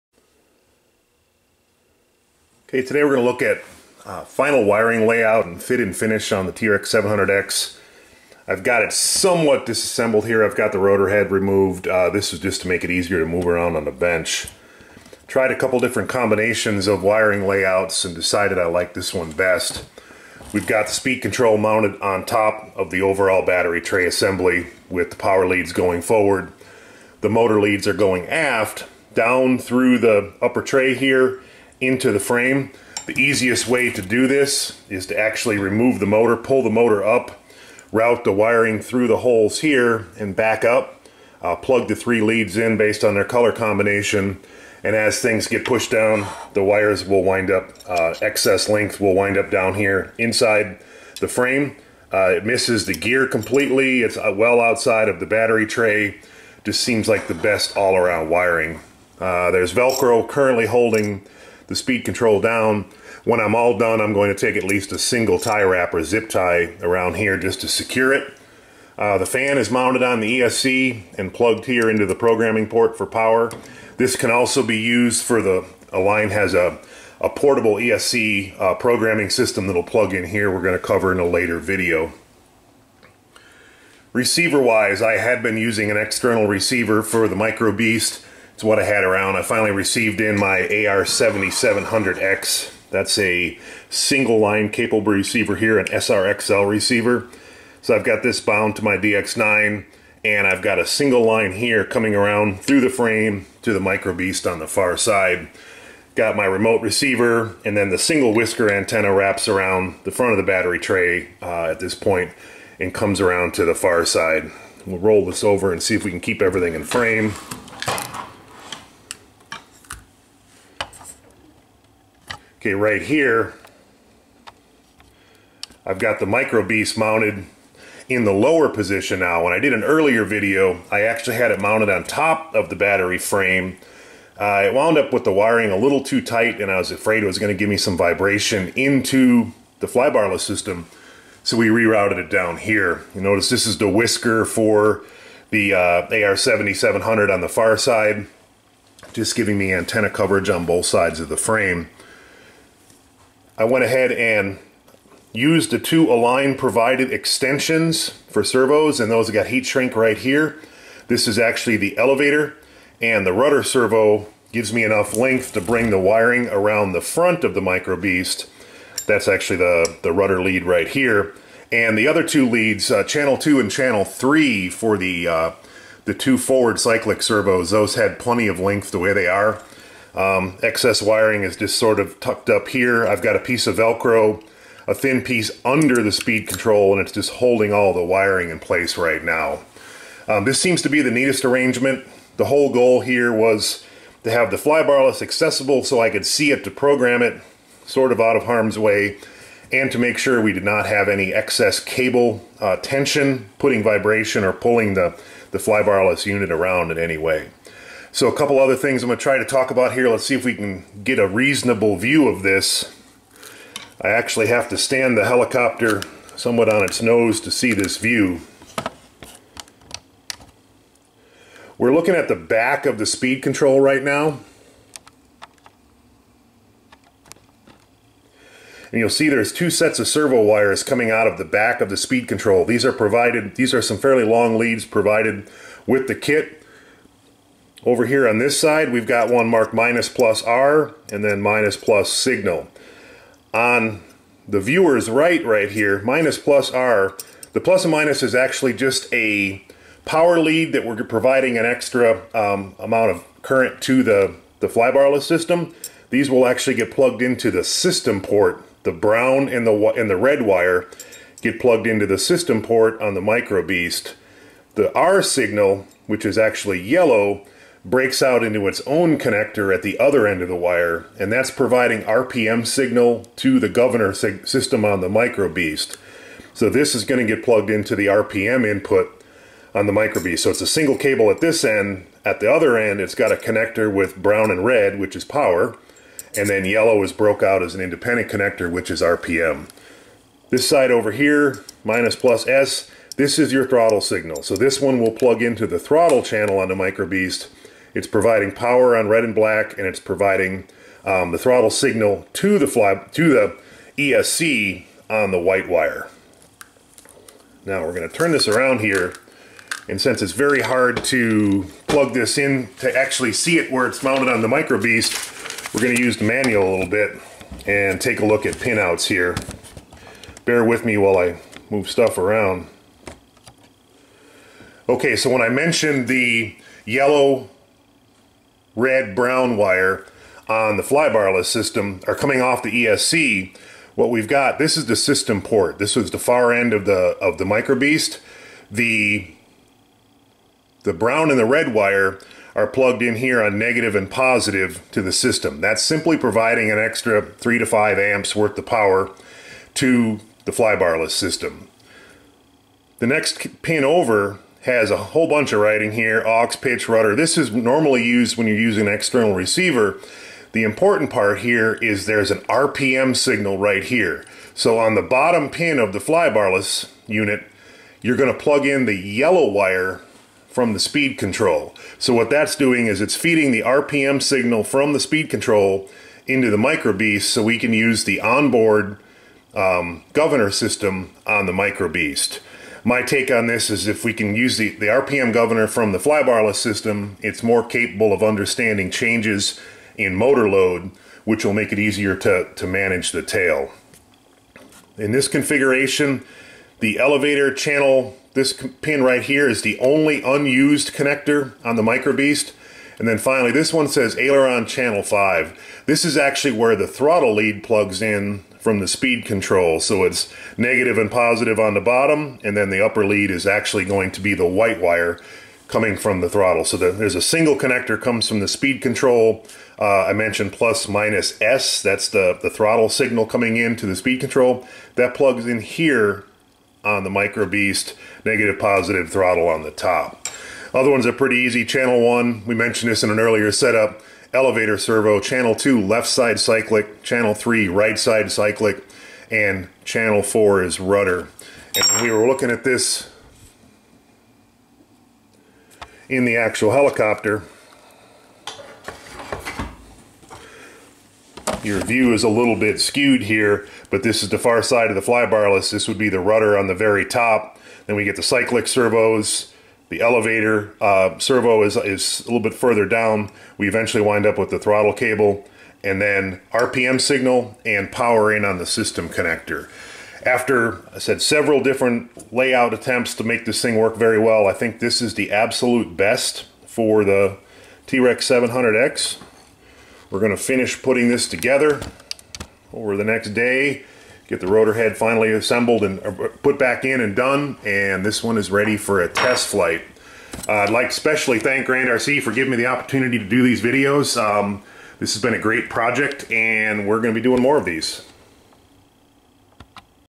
TREX700X Final wiring setup
Final wiring config for motor, ESC and MicroBeast on TREX700X